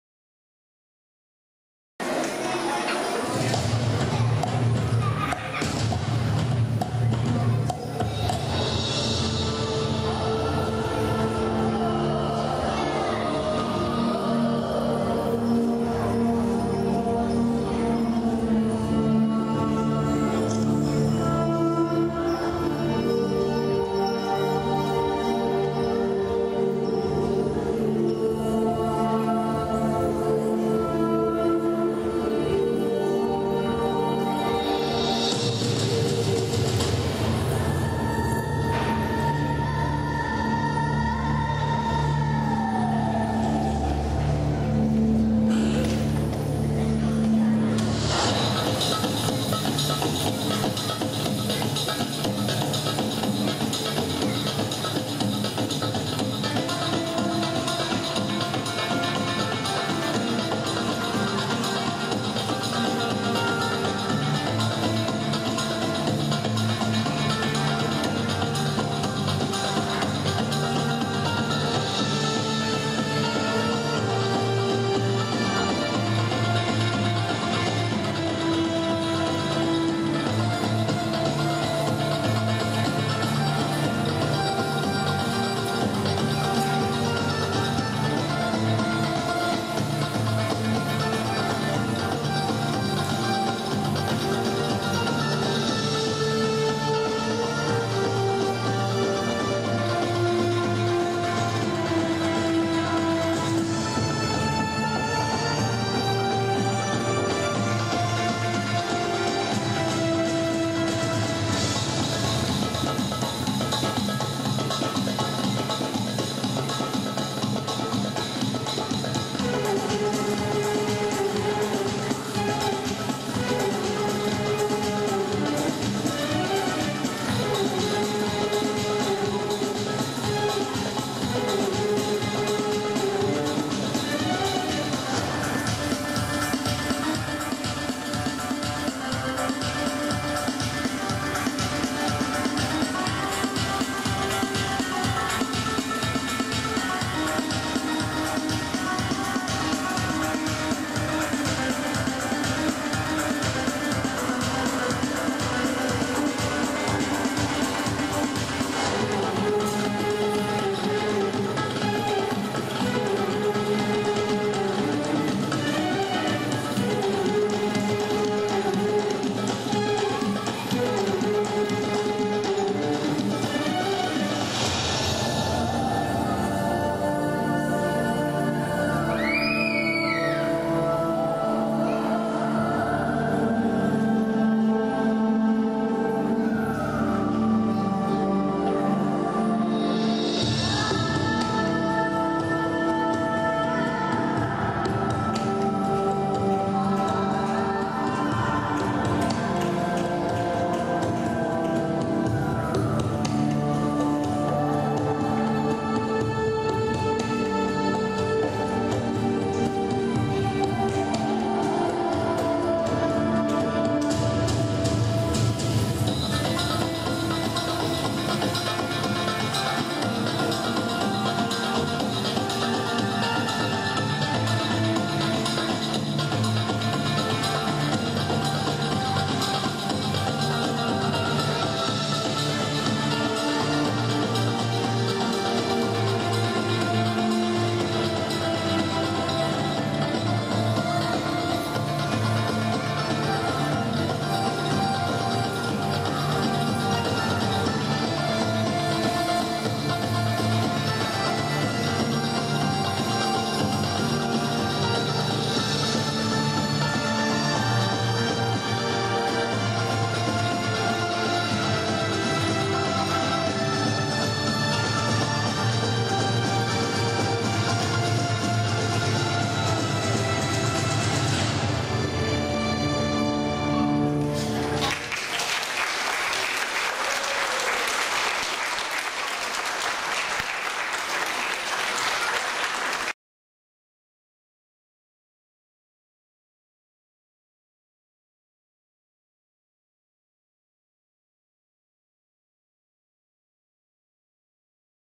Závěrečné vystoupení 2012, Břišní tance zač. výběr
Závěrečná vystoupení tanečního oddělení DDM Praha 6, Sál na Marjánce 7.6.2012,
Břišní tance zač. / výběr, Tanec s křídly - Ice Queen / Ledová královna. Lektorka: Kateřina Hlinková